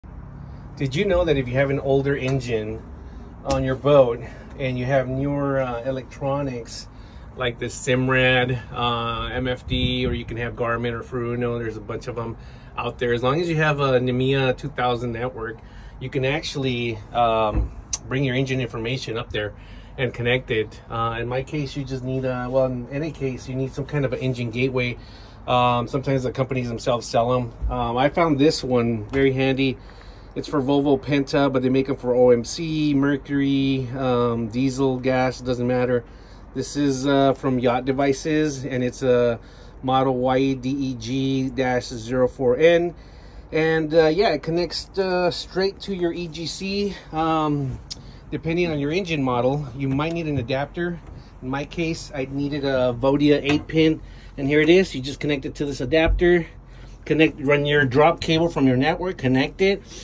Is your boat engine connected to your MFD / Chartplotter?
Easily connect your inboard or outboard engine to your Simrad, Garmin or Furuno MFD / Chartplotter using an engine gateway to get your engine data to your NMEA 2000 network.  In this video I used the YDEG-04 from Yatch Devices and a Vodia 8-pin adapter cable to connect my Volvo Penta 5.7 GI-F to my Simrad NSS EVO3 12's.

Now I'm getting alternator, voltage, engine hours, fuel rate, oil pressure, RPM's, Engine temp and Torque on my MFD.